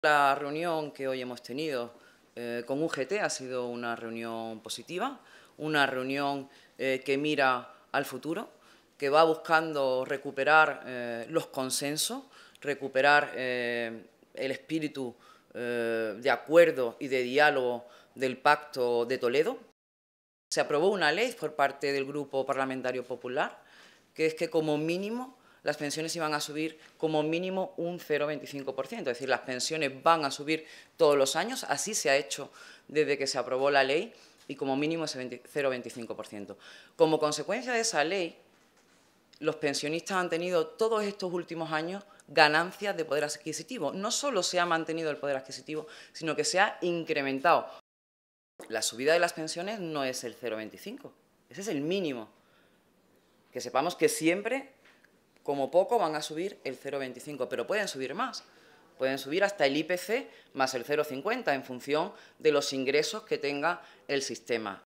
Carolina España tras la reunión de UGT
La portavoz del GPP en la Comisión del Pacto de Toledo, Carolina España, ha destacado hoy que gracias a la ley que aprobó el Ejecutivo de Mariano Rajoy, “durante los últimos años los pensionistas siempre han ganado poder adquisitivo”.

Según ha explicado la portavoz popular, “esta Ley fija que, como mínimo, las pensiones siempre suban un 0,25%, y como estos últimos años el IPC ha sido negativo todos los pensionistas han ganado poder adquisitivo”. Además, ha enfatizado que “esta norma establece que, como mínimo, se suban las pensiones un 0,25%, pero en función de los ingresos del sistema se pueden subir hasta un 0,5%+IPC”.

“Todos los años por estas fechas, la oposición decía que había que aplicar el IPC como referencia para actualizar las pensiones, pero si les hubiéramos hecho caso, las pensiones se hubieran tenido que reducir y los pensionistas hubiesen perdido poder adquisitivo porque el IPC estaba en negativo”, ha subrayado Carolina España, quien ha recordado que  “hoy, el IPC sigue por debajo de ese 0,25%”.